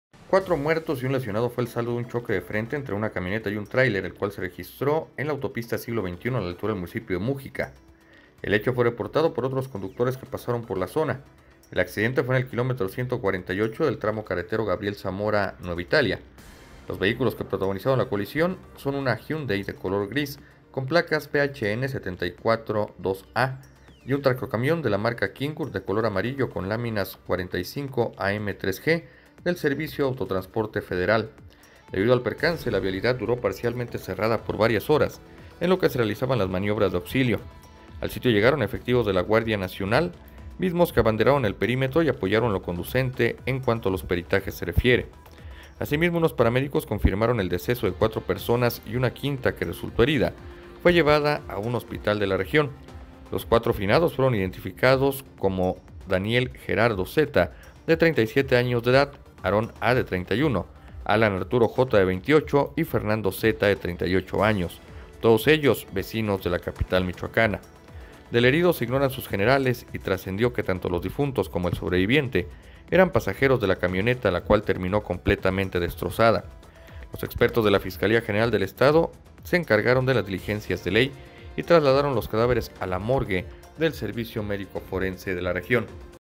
4 muertos y un lesionado al chocar de frente su camioneta contra un tráiler en la Siglo XXI
4 muertos y un lesionado al chocar de frente su camioneta contra un tráiler en la Siglo XXI

Múgica, Mich.- Viernes 27 de agosto de 2021.-  Cuatro muertos y un lesionado fue el saldo de un choque de frente entre una camioneta y un tráiler, el cual se registró este viernes en la autopista Siglo XXI, a la altura de este municipio de Múgica, de acuerdo con la información recabada por esta agencia de noticias en la labor periodística.

El hecho fue reportado por otros conductores que pasaron por la zona. El accidente fue en el kilómetro 148+500 del tramo carretero Uruapan-Lázaro Cárdenas. Los vehículos que protagonizaron la colisión son una Hyundai, de color gris, con placas de circulación PHN742A y un tractocamión de la marca Kenworth, de color amarillo, con láminas 45AM3G del Servicio de Autotransporte Federal.

Debido al percance la vialidad duró parcialmente cerrada por varias horas en lo que se realizaban las maniobras de auxilio. Al sitio llegaron los efectivos del Batallón de Seguridad en Carreteras e Instalaciones de la Guardia Nacional, mismos que abanderaron el perímetro y apoyaron en lo conducente en cuanto los peritajes se refiere.

Asimismo, unos paramédicos locales confirmaron el deceso de cuatro personas y a una quinta que resultó herida la llevaron a un hospital de la región. Los cuatro finados fueron identificados gracias a sus credenciales del Instituto Nacional Electoral y así se supo que respondían en vida a los nombres de Daniel Gerardo Z., de 37 años de edad, Aarón A., de 31 años de edad, Alán Arturo J., de 28 años de edad y Fernando Z., de 38 años de edad, todos ellos domiciliados en la ciudad de Morelia.

Del herido se ignoran sus generales y trascendió que tanto los difuntos como el sobreviviente eran pasajeros de la camioneta Hyundai, la cual terminó completamente destrozada. Los expertos de la Fiscalía General del Estado de Michoacán se encargaron de las diligencias de ley y del traslado de los cadáveres al Servicio Médico Forense.